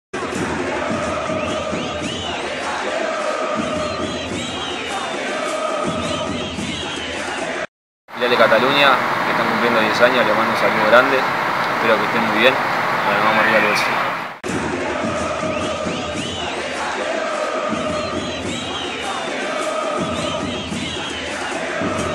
Saludo Carlos de Pena - 10 años de la Filial Catalunya de Nacional
Carlos de Pena salto desde la Cantera al primer equipo de Nacional donde permanecio desde el año 2013 hasta el año 2015 consiguiendo los siguientes titulos:

Muchisimas gracias a Carlos de Pena !!!